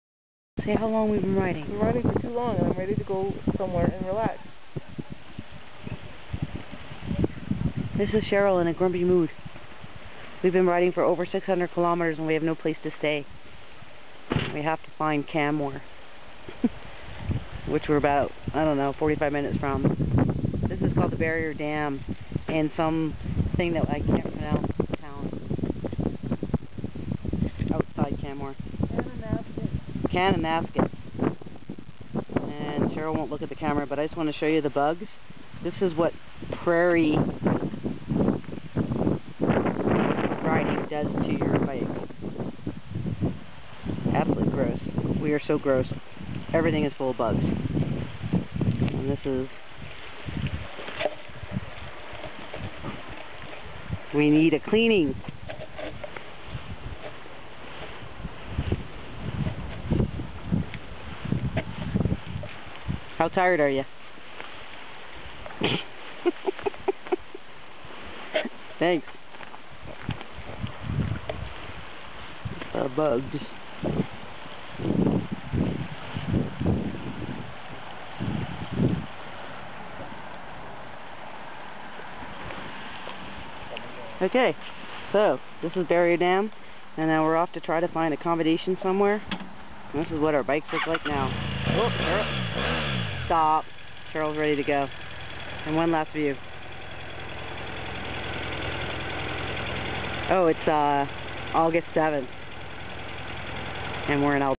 BMW 2010 F650GS - Cross Country Tour Barrier Dam, Kananaskis, Alberta
August 7th 2010 - A quick stop at the Barrier Dam on our way home from our cross country trip.  This was day 22 I think!